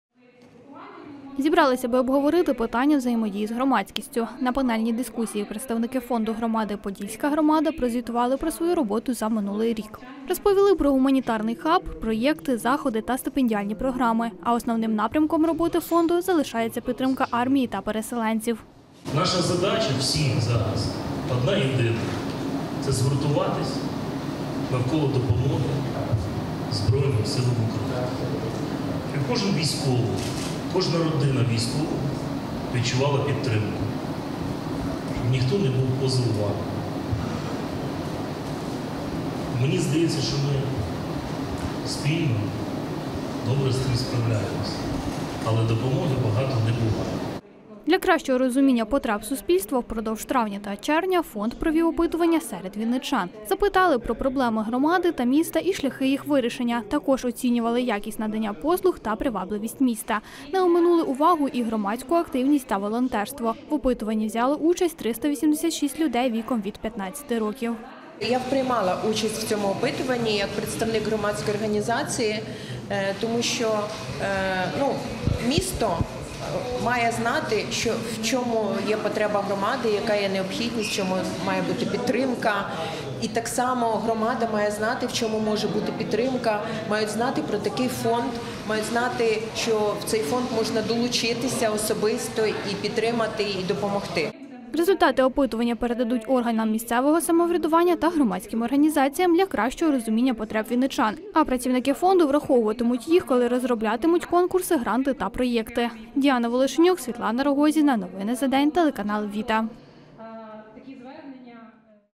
Від потреб до дій: на панельній дискусії обговорили потреби суспільства
Зібрались аби обговорити питання взаємодії з громадськістю. На панельній дискусії представники фонду громади "Подільська громада" прозвітували про свою роботу за минулий рік. Розповіли про гуманітарний хаб, проєкти, заходи та стипендіальні програми. А основним напрямком роботи фонду залишається підтримка армії та переселенців. Для кращого розуміння потреб суспільства впродовж травня та червня фонд провів опитування серед вінничан. Запитували про проблеми громади та міста, і шляхи їх вирішення, також оцінювали якість надання послуг та привабливості міста. Не оминули увагою і громадську активність та волонтерство. В опитуванні взяли участь 386 людей, віком від 15 років.